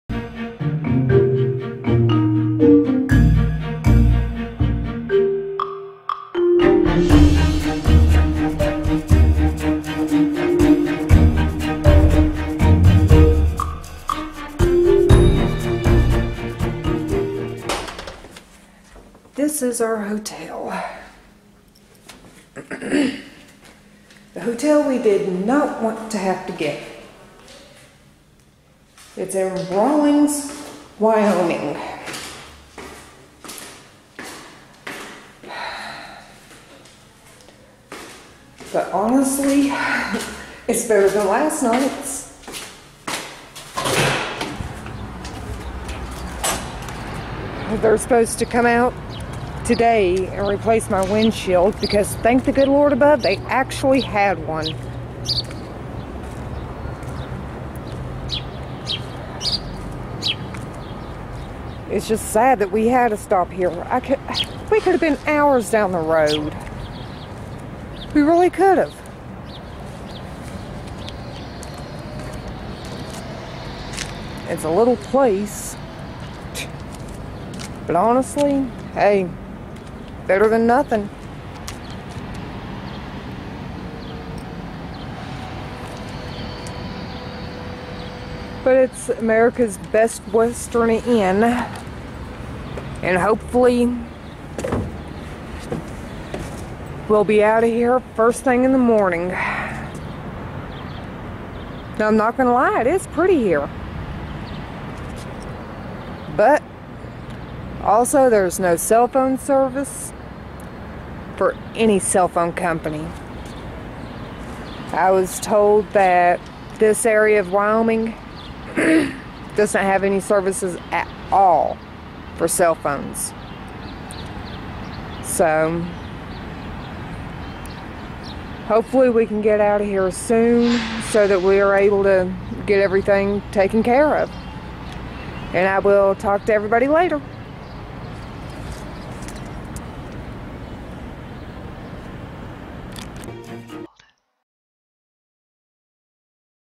Should you buy offgrid property on Ebay? Nightmare or dream? Tour outside Wyoming hotel
This is the video diary  of our almost 5,000 mile road trip from WV to Nevada to look at property that we have invested several thousand dollars in sight unseen. Is investing in property on Ebay a good idea or not? We will let you know as you travel along with us on our trip. So relax and join us before you buy or if you are just curious.